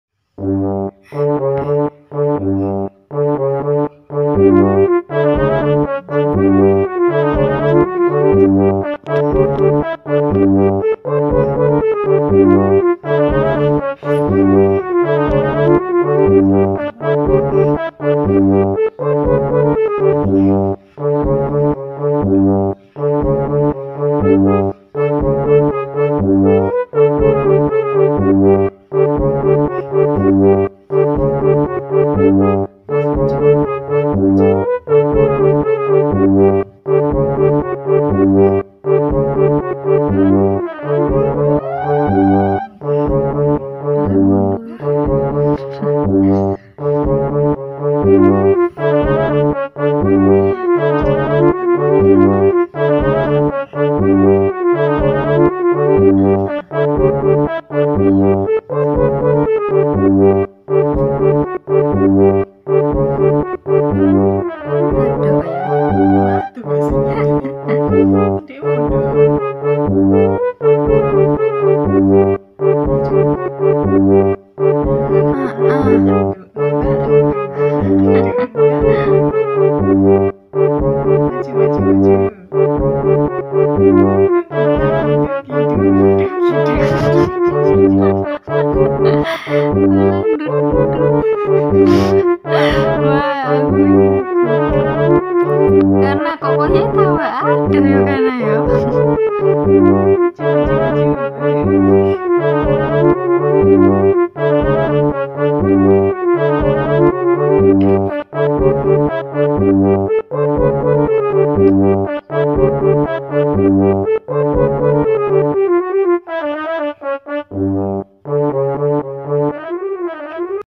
338. Adekk Chana umur 5 bulan sudah bisa onggong-onggong 👏👏
MasyaAllah Tabarakallah.. jadi anak bayi itu berat ya, dia harus belajar banyak hal. tetap apresiasi setiap tumbuh kembangnya, dan jangan pernah membandingkan anak kita dengan anak orang lain! karena setiap tumbuh kembang anak itu beda2 dan bervariasi..